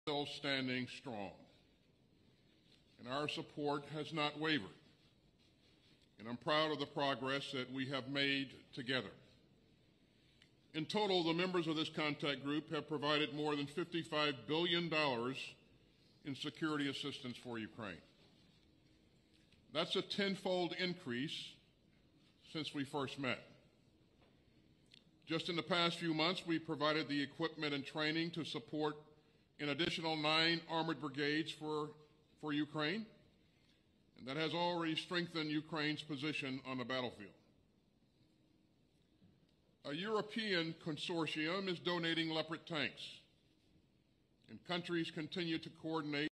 A meeting of the Contact Group on Defense of Ukraine is held at the Ramstein Air Base.
Ukraine hopes that its strategic partners will continue to support it in every possible way.